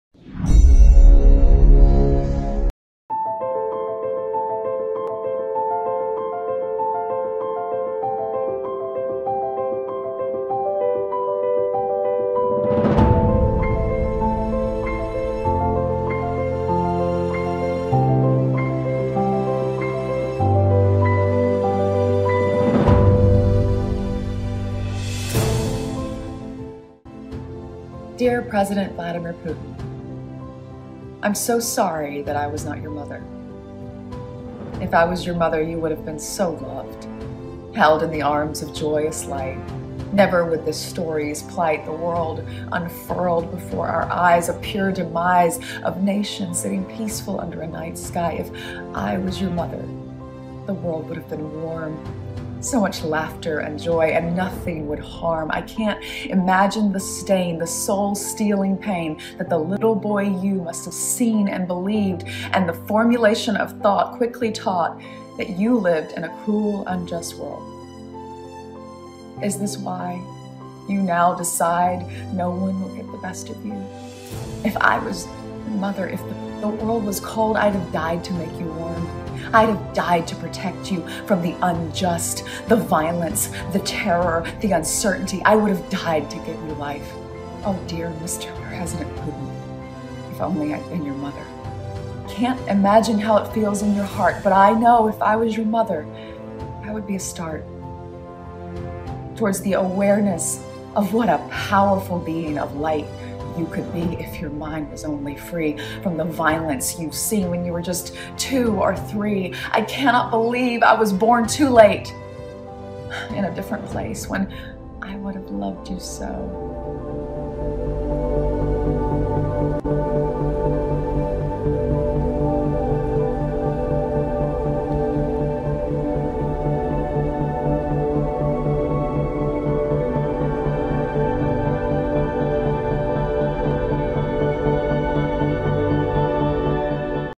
Buat Puisi untuk Putin, Artis Annalynne McCord Di Cibir Netizen
Aktris Amerika Serikat Annalynne McCord mengunggah puisi untuk Presiden Rusia, Vladimir Putin. Sayangnya niat Annalynne mendapat komentar tidak menyenangkan dari netizen
Selamat menyaksikan video Buat Puisi untuk Putin, Artis Annalynne McCord Di Cibir Netizen ini. terimakasih